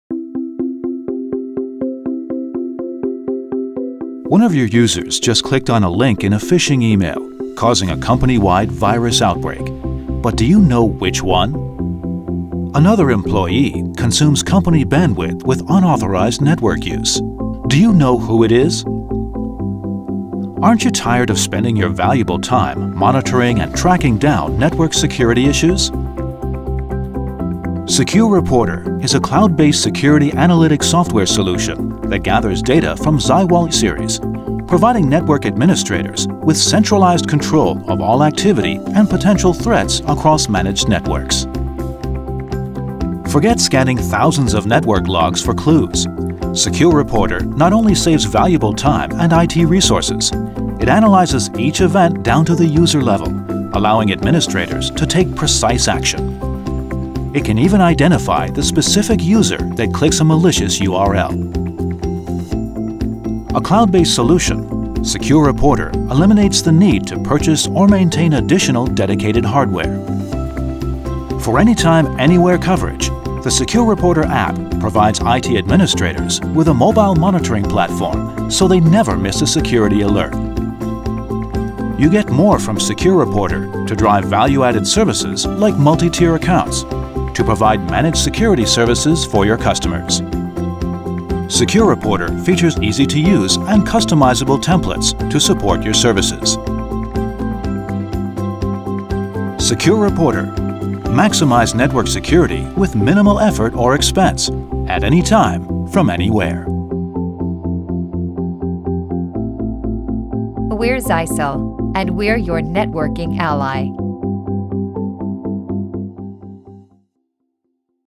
Zyxel SecuReporter Bulut Ağ Güvenliği Analytics Hizmeti
Zyxel SecuReporter, ZyWALL ürün serileri için tasarlanmış toplama ve korelasyon özellikleri ile bulut tabanlı akıllı analitik ve rapor servisidir. Ağ yöneticilerine, ağ faaliyetleri ve tüm ağdaki potansiyel tehditler hakkında merkezi bir bakış açısı sağlar.

• Bulut tabanlı “Hizmet Olarak Yazılım” (SaaS)
• Kesin derinlemesine analiz
• Özel ve önceden yapılandırılmış raporlama
• Kurulum ve izleme kolaylığı
• App anlık uyarılar ve bildirimler (2019 ilk çeyreğinde mevcut app)